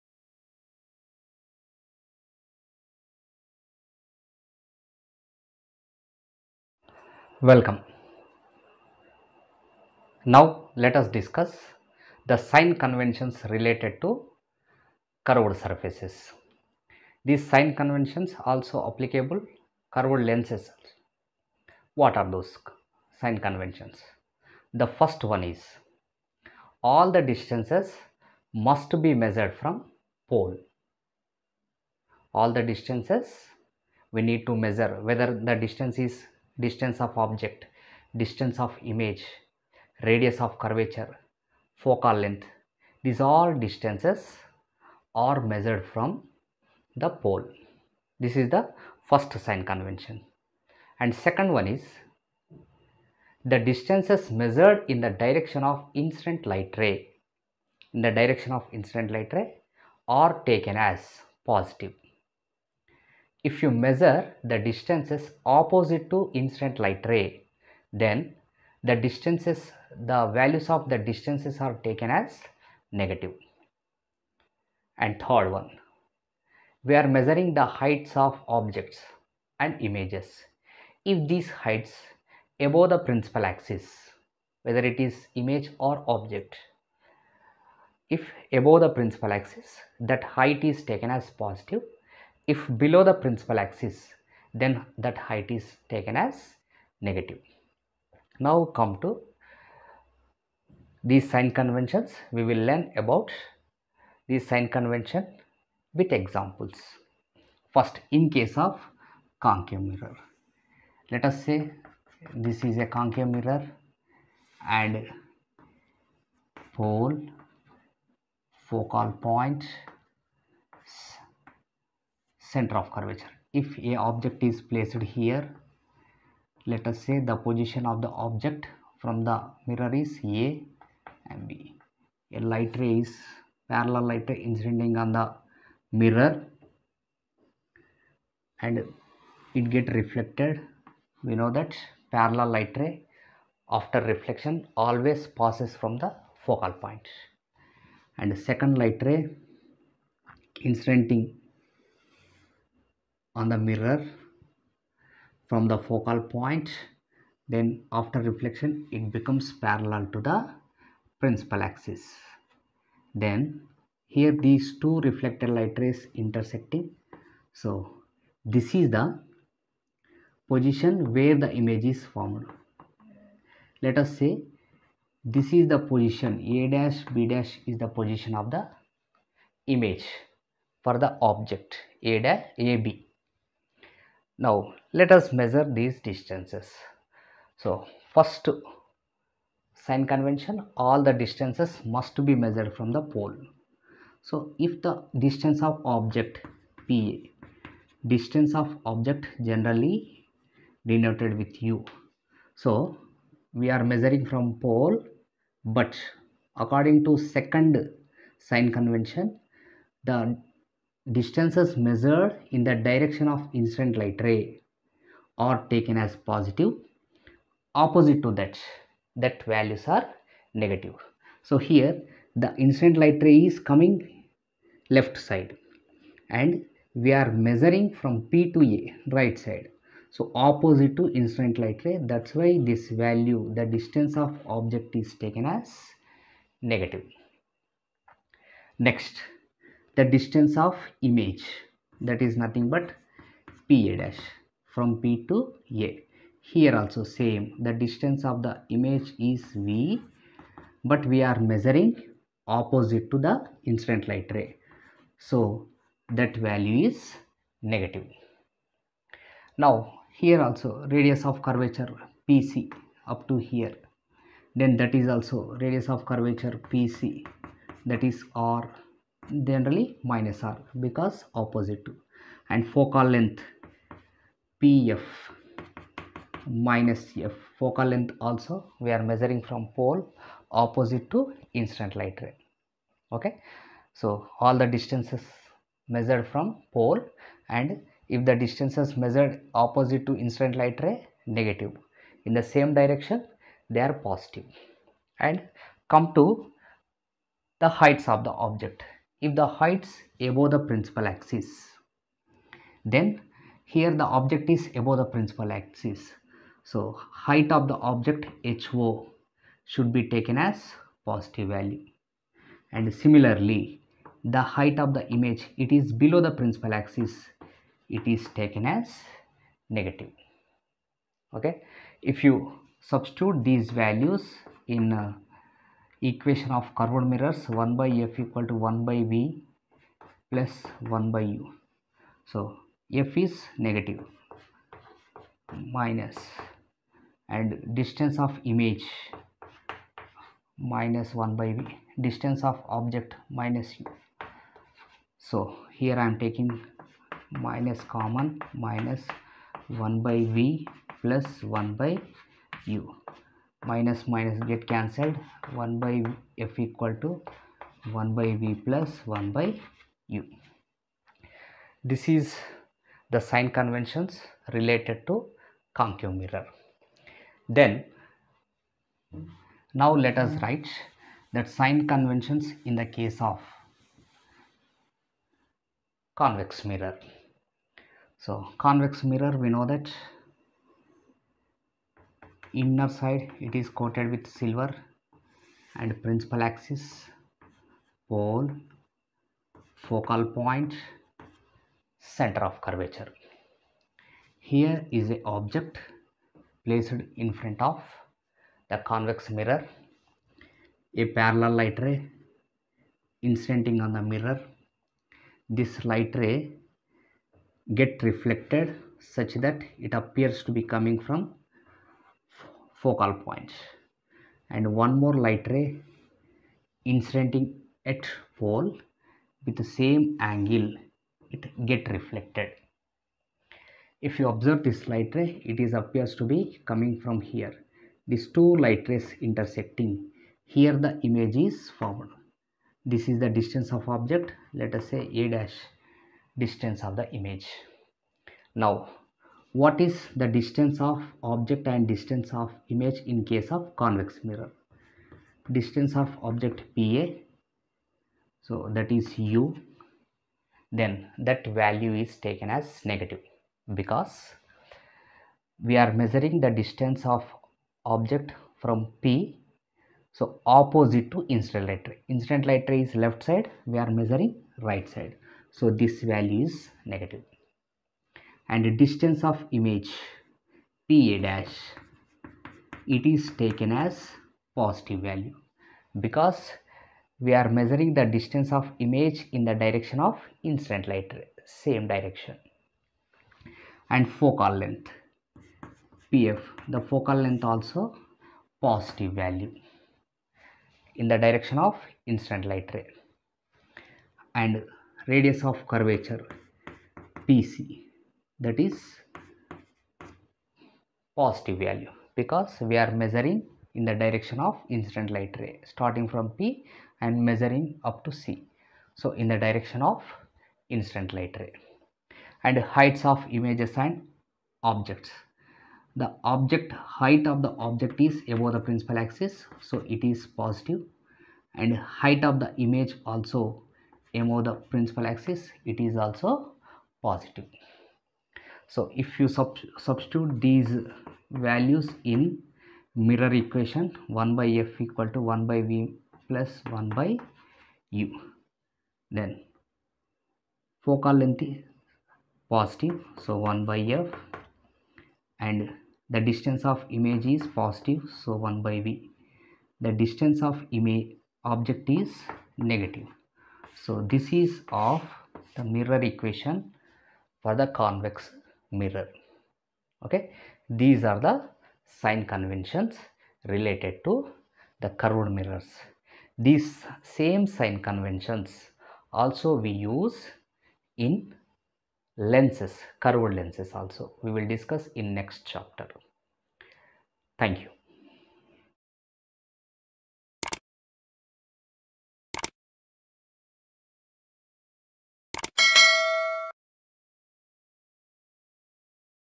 Sign Convention | Sign Convention parameters for Curved Surfaces Class 10 | Mirror Equation Signs
**Physics Learning Point**

Sign Convention parameters for Mirror Equation is also applicable for all Curved Surfaces including Lenses.
Online Physics class.
How to write sign Convention parameters for Curved Surfaces?
How to measure Radius of Curvature, focal length, distance of image, distance of Object using sign Convention?
Reflection of Light at Curved Surfaces.
Class 10 physics.
CBSE syllabus.